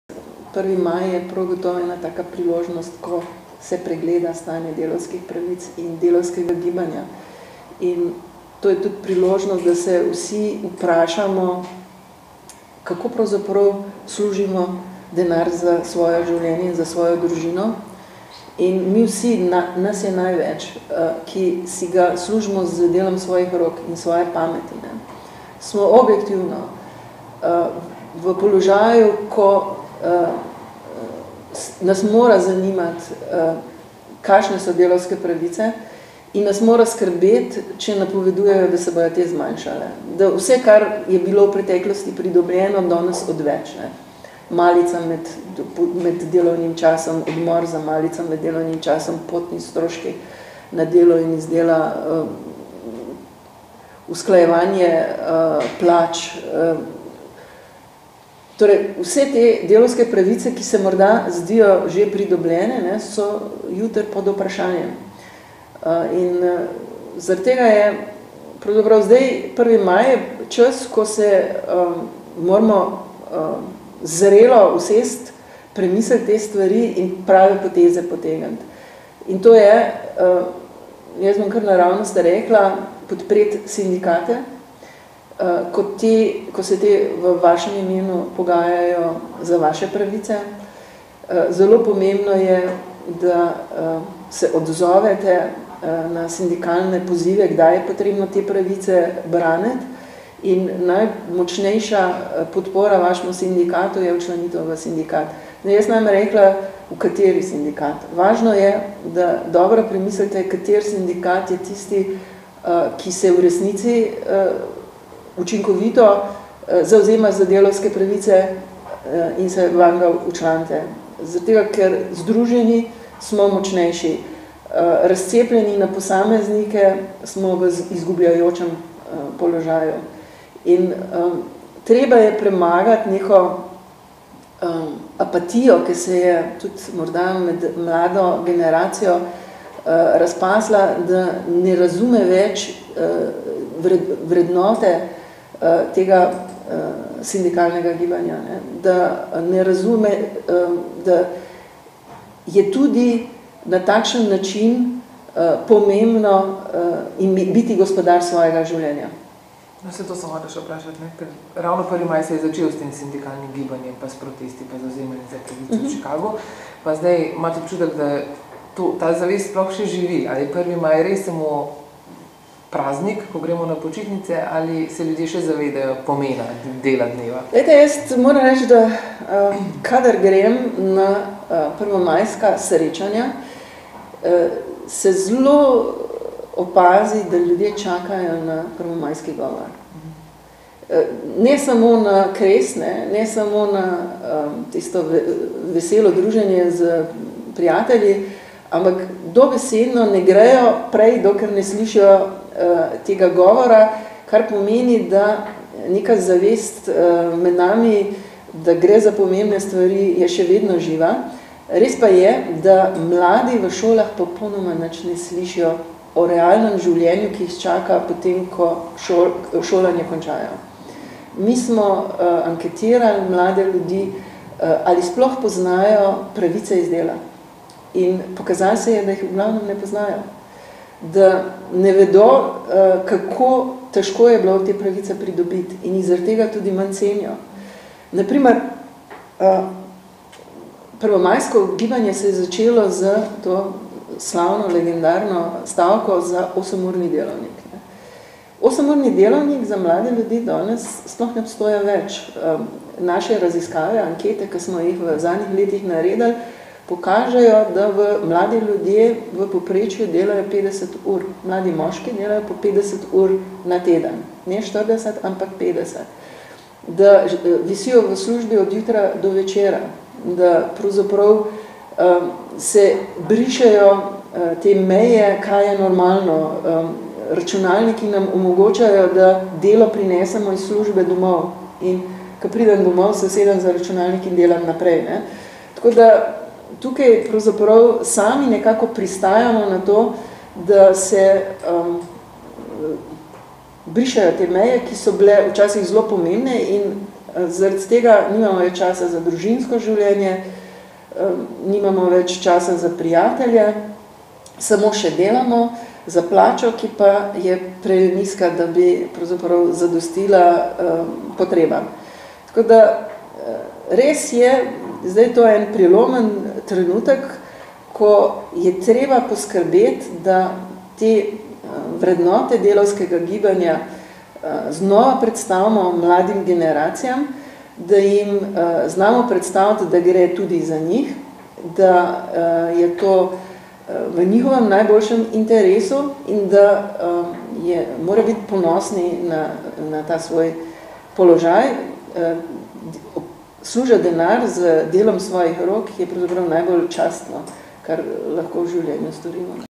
Lučka Böhm, izvršna sekretarka ZSSS o prvem maju (vest.si)
Izvršna sekretarka ZSSS o prvem maju in s programom prireditev ob delavskem prazniku.